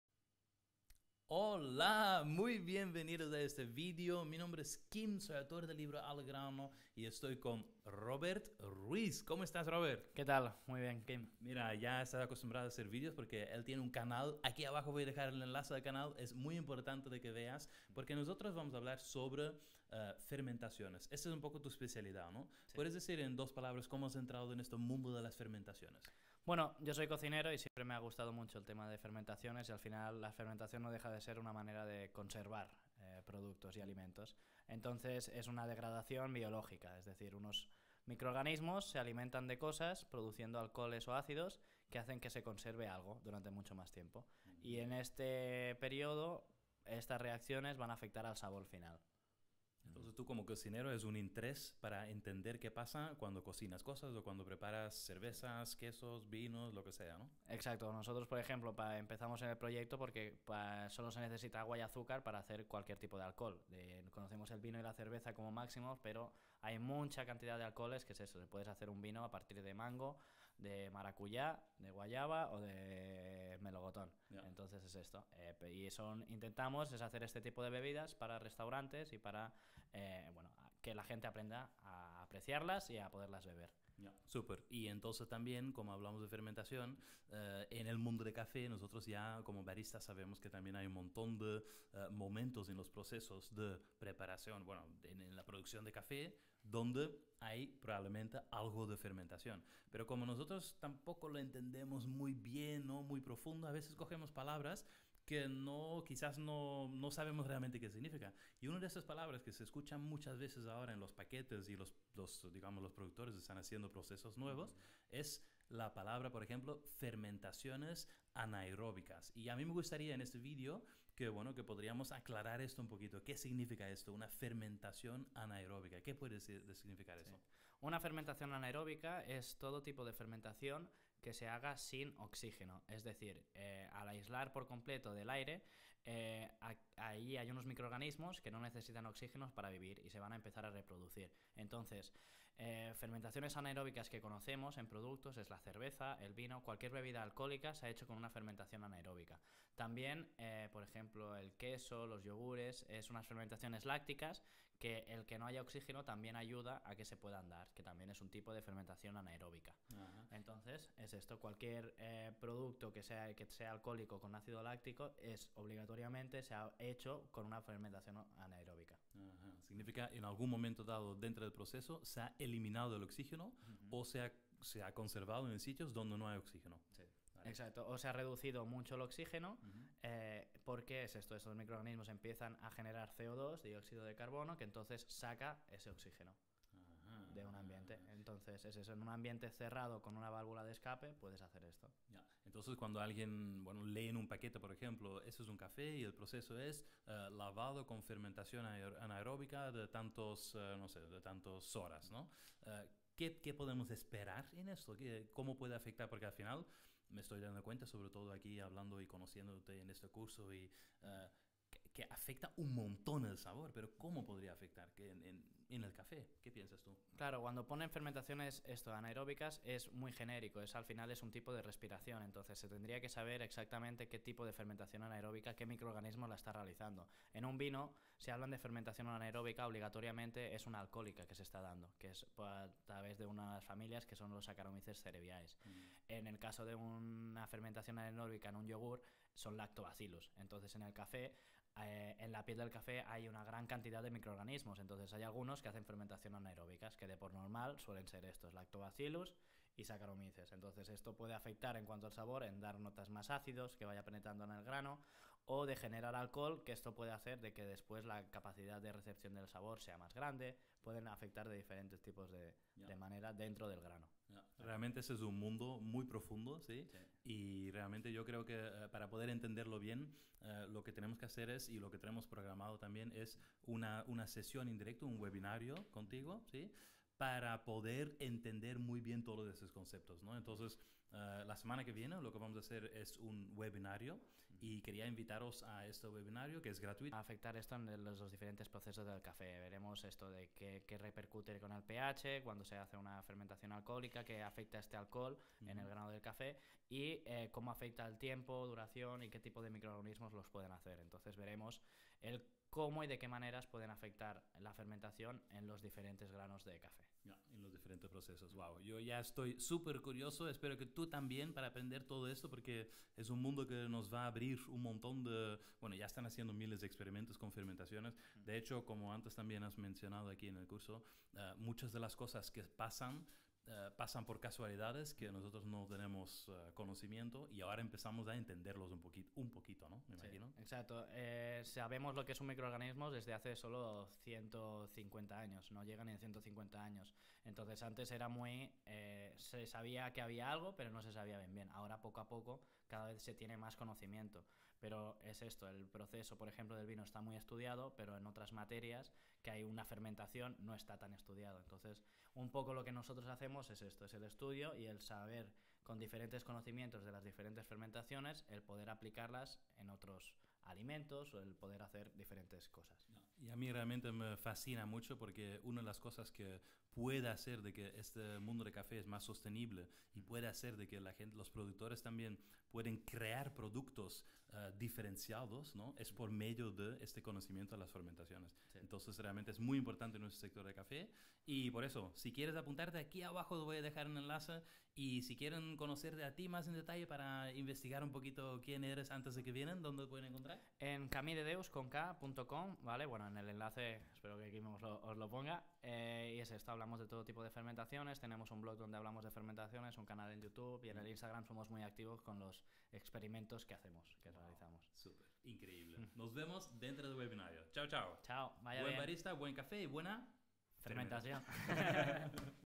Cómo afecta la fermentación al café, te invitamos al webinario
Buenas amigos!! Os invito a poder ver de que manera puede afectar la fermentación al café.
Este viernes 9 de mayo realizaremos un webinario com Kim Ossenblock el gran maestro cafetero, autor de el libro Al grano!
En este webinario profundizaremos sobre como puede afectar la fermentación al café.